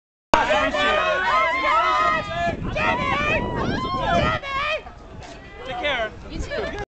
Jimmy and Chantal walking to the bus
Jimmy Urine and Chantal Claret heading back to the bus after the show in Philadelphia May 2nd, 2008.

Be sure to visit the Nation Of Crackers forum for latest news, info, and other fans.